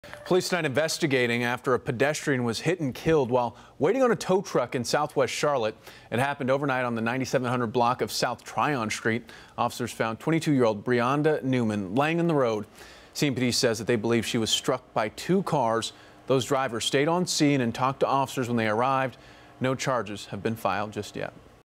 Pedestrian struck and killed in southwest Charlotte
CMPD says they believe she was struck by two cars.
Those drivers stayed on scene and talked to officers when they arrived.